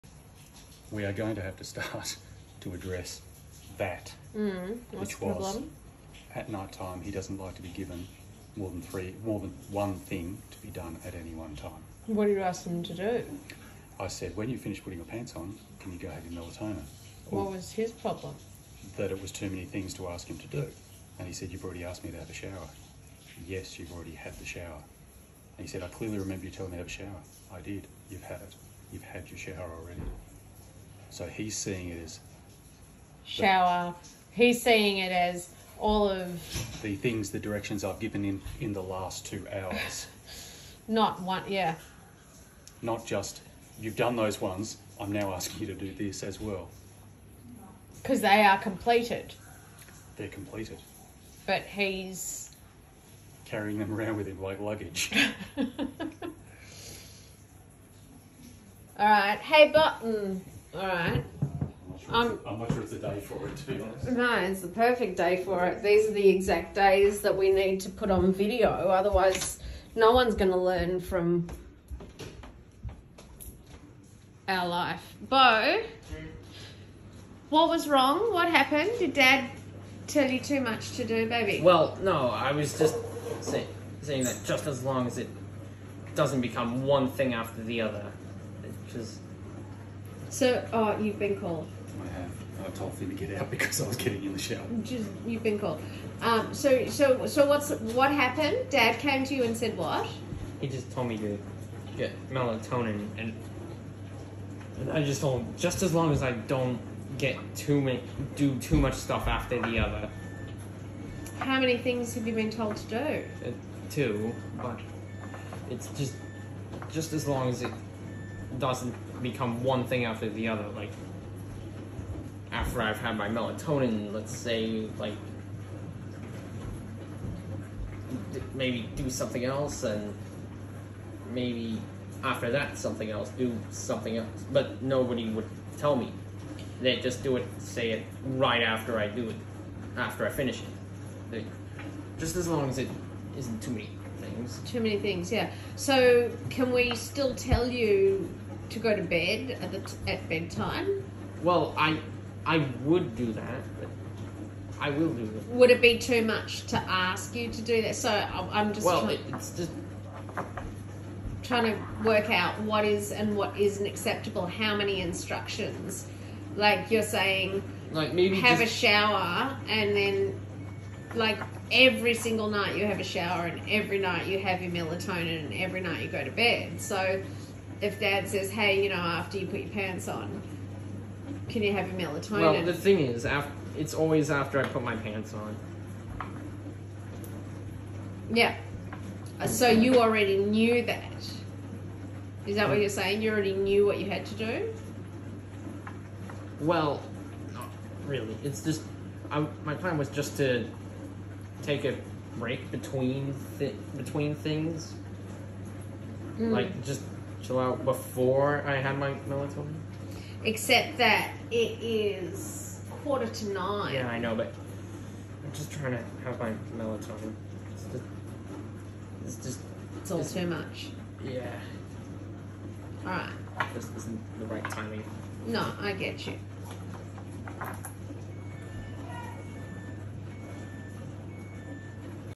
Overwhelmed
According to general practitioners children reach certain milestones at certain ages by the age of about 1 1/2 they should be able to complete a task mission, then three task and so on until you can tell someone go to the end of the road pick up a stone bring it back here and put it on the table… This is very rarely the case with autism and one of the biggest things I noticed about Beau when he was young. At nearly 16 we are still struggling with two tasks at a time￼